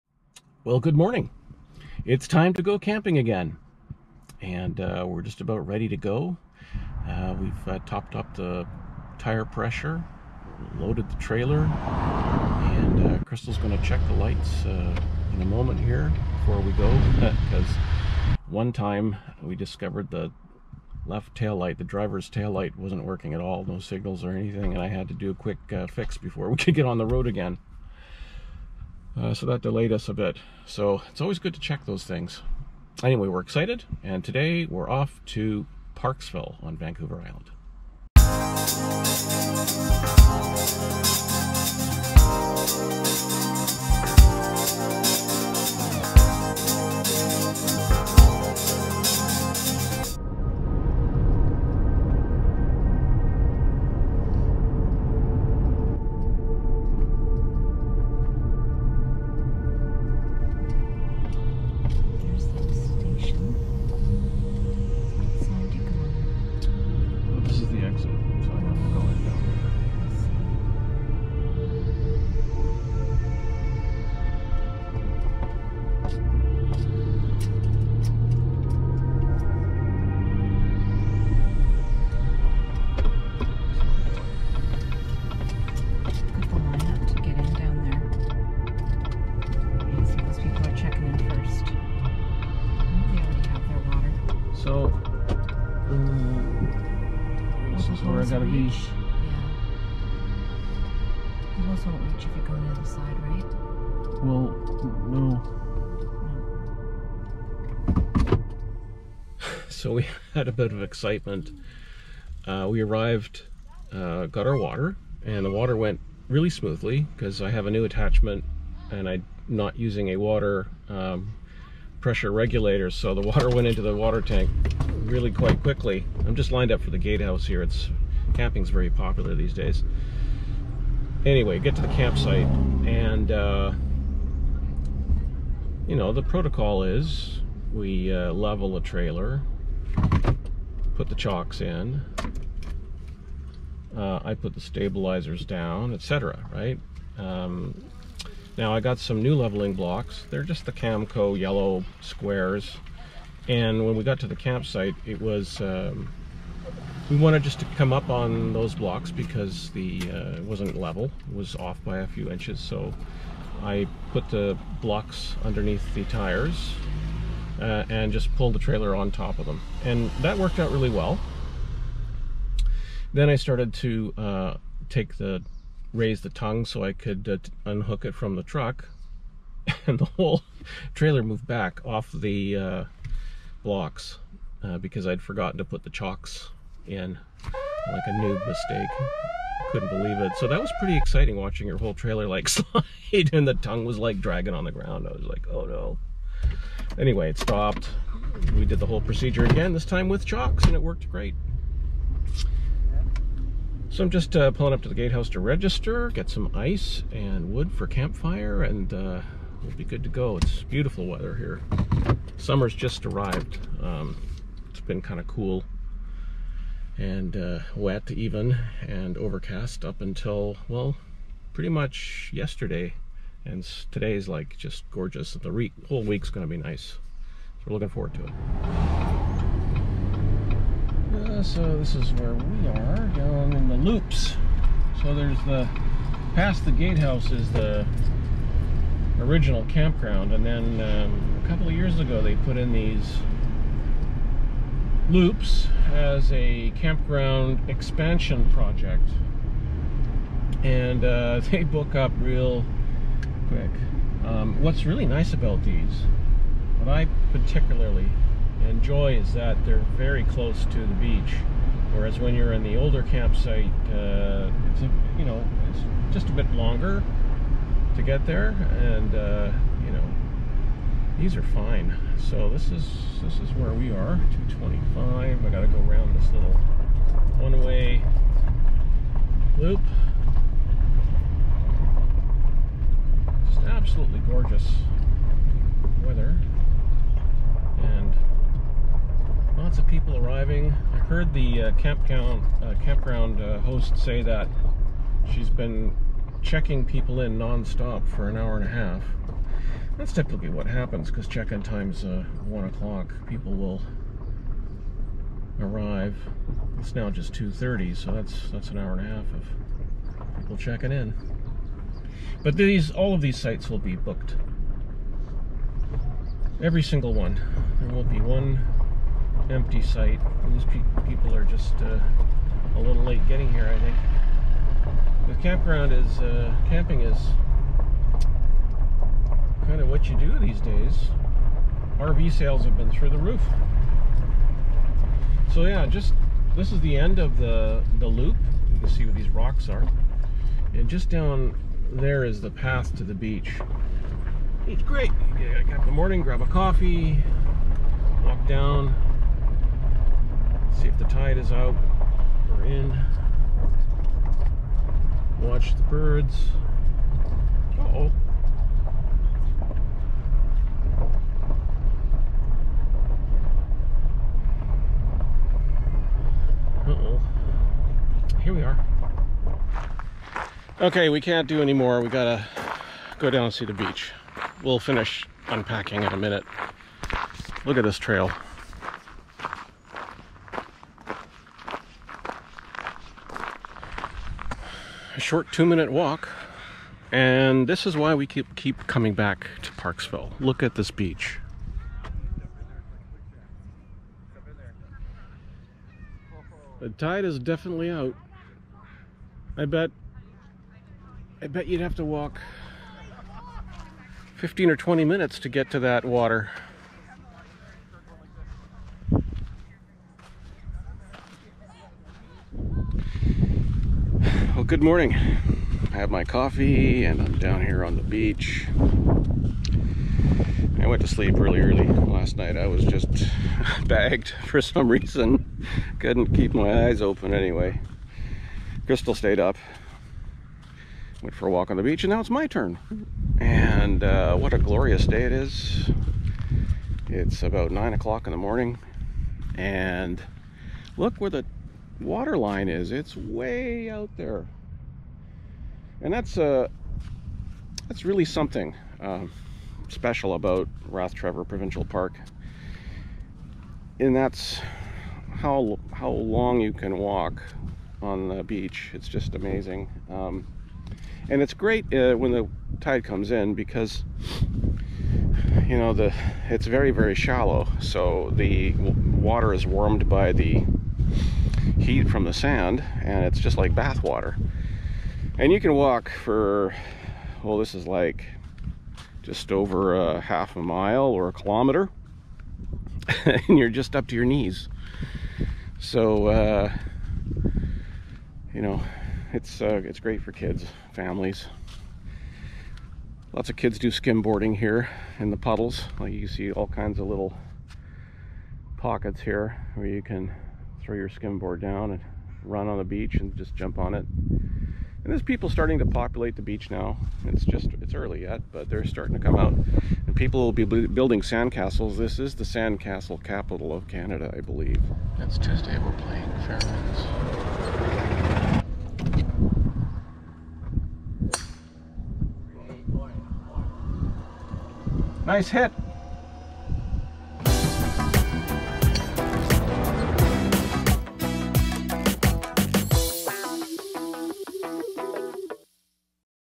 Rathtrevor Provincial Park, Vancouver Island, British Columbia 2020
A brief look at our camping trip to Rathtrevor Provincial Park.